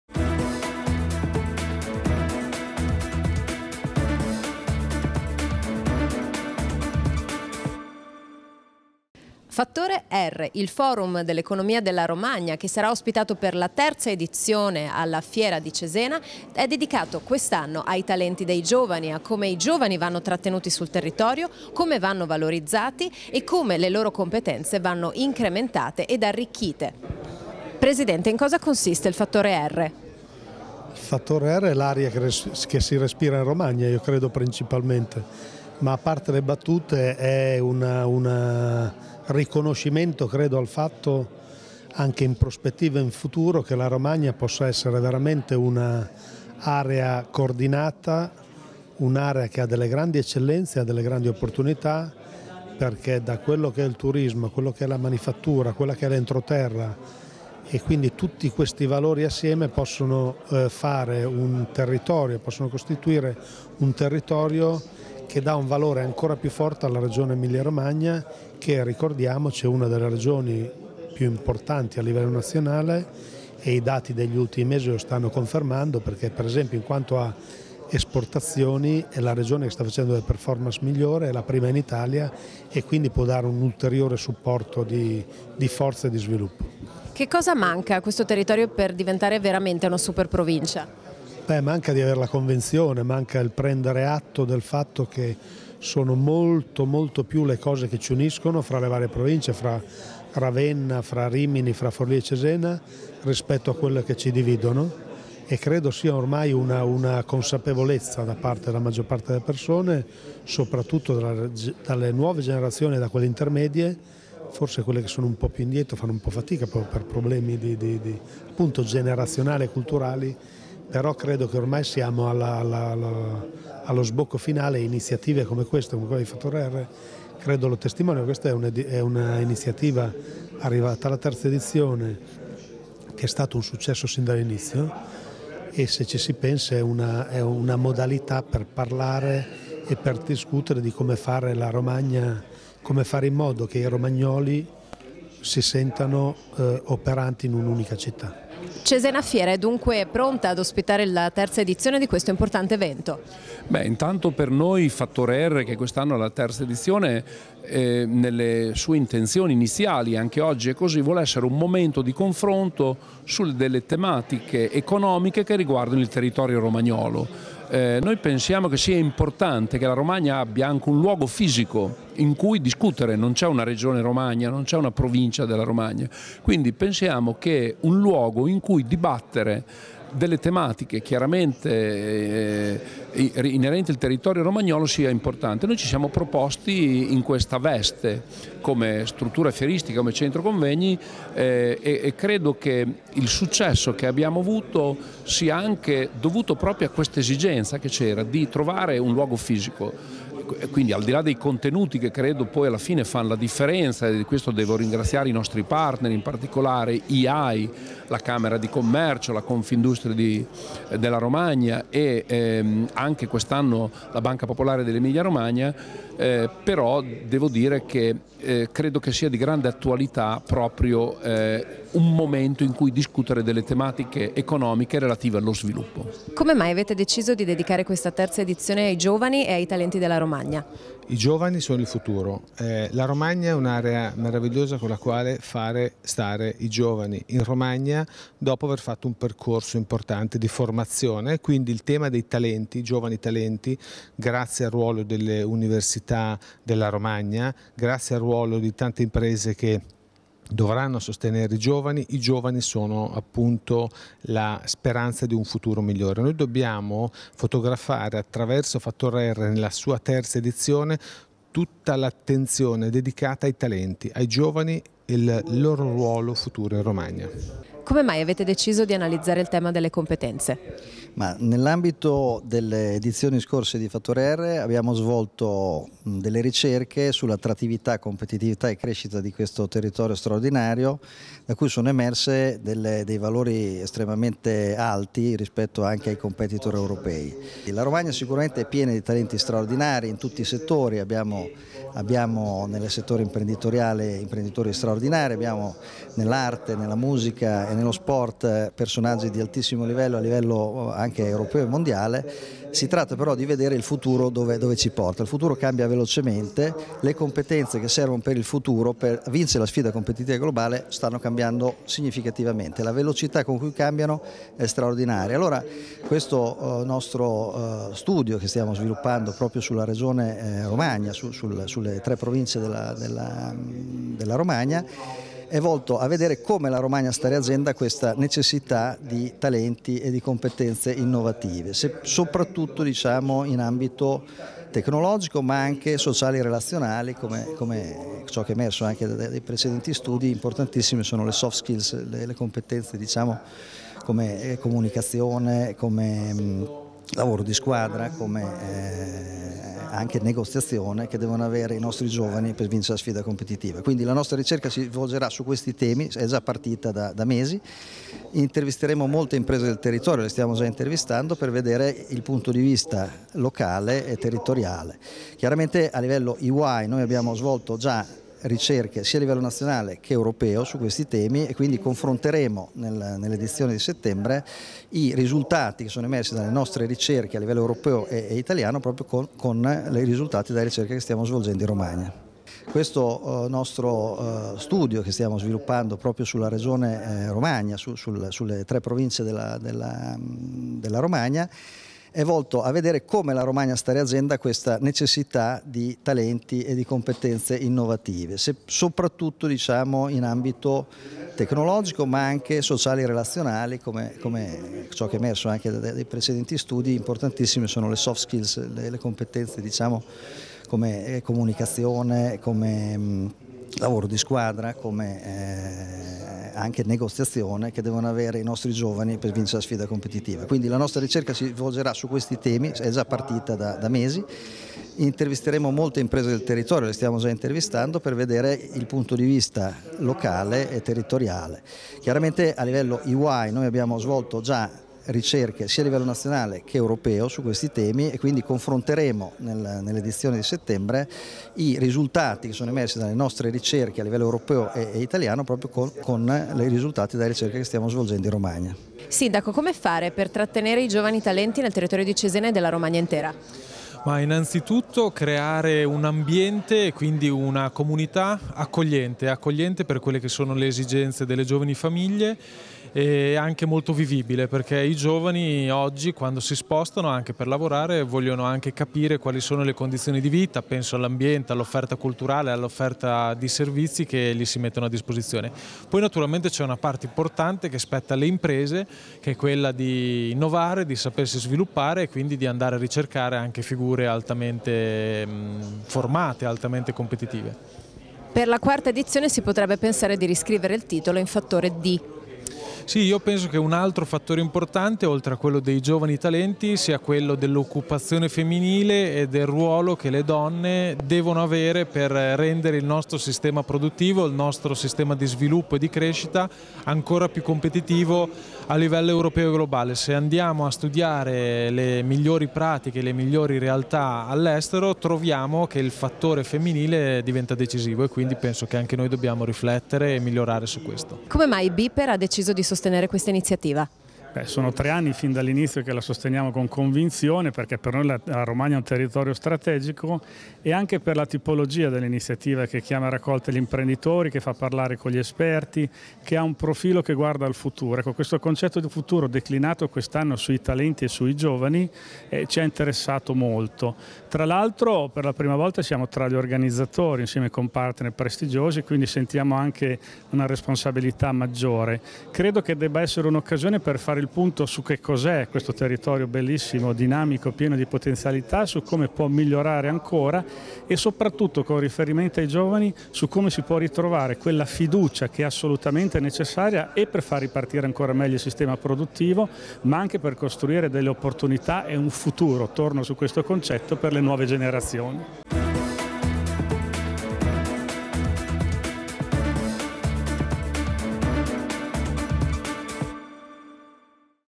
CESENA: Giovani e talenti al centro della terza edizione di "Fattore R" 24-07-2019
CESENA: Giovani e talenti al centro della terza edizione di "Fattore R"